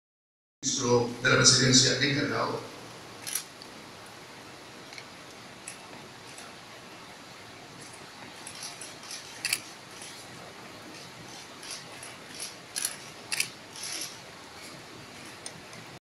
Toma de posesión de Ángela Russo como magistrada de la CSJ
Momentos en que Ángela Russo firma oficialmente en la Presidencia de la República.